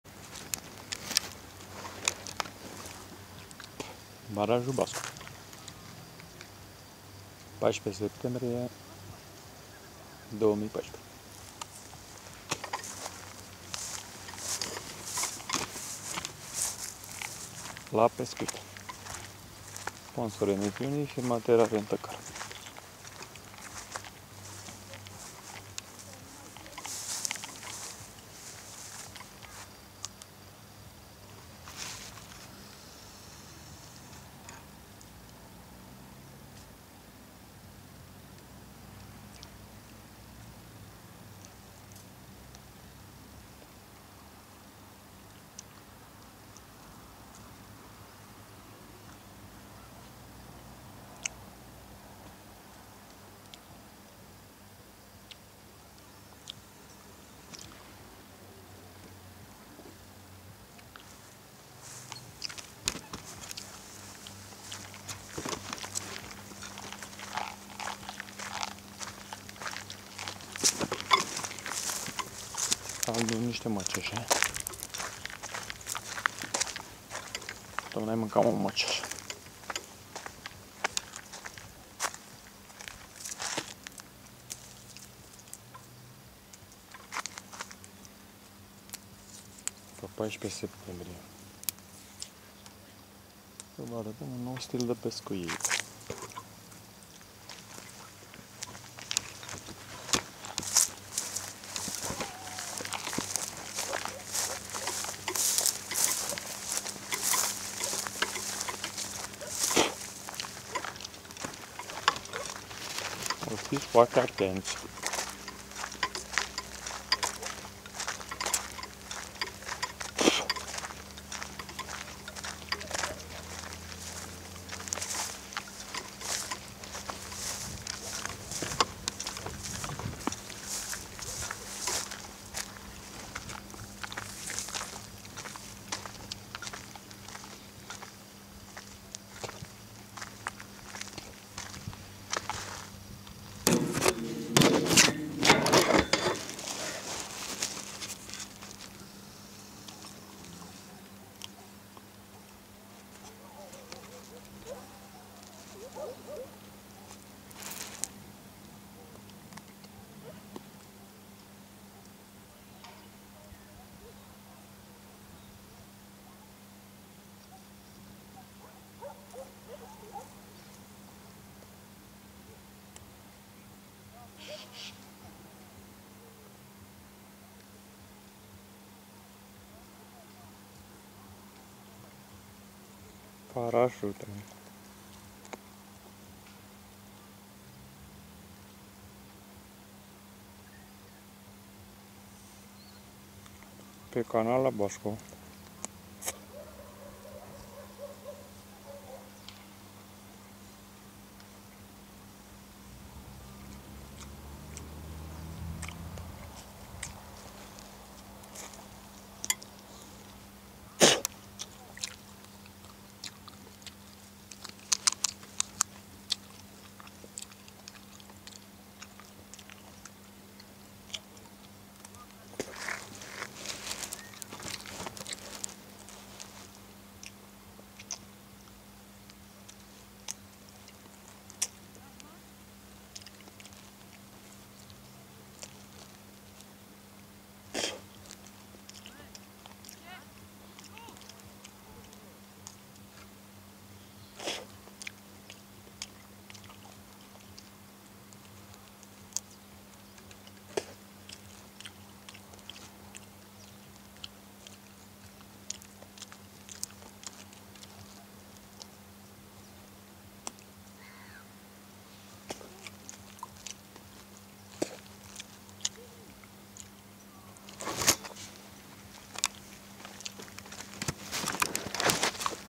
Braconaj piscicol la barajul Bascov,septembrie 2014
Locatia unde se da cu parasuta si se braconeaza este  barajul Bascov,dar acestia sint ,,palmasii" cei care fac dezastrul sint ,,electricienii" ,adica cei care prind pestele cu ajutorul firelor de curent electric .Si mai nou am auzit ultima bomba in materie de braconaj,tineti-va bine:Butelia de la aragaz,va vine sa credeti?.Adica se duc braconierii cu butelia de la aragaz si ii da drumul la robinet pe deschis si o arunca in apa.Butelia se duce la fundul apei fiind grea si gazul din ea iese in apa,pestii daca nu mai au oxigen in apa ,mor, si butelia dupa ce se descarca iese la suprafata apei.Nu stie dracul cum sa mai distruga pamintul.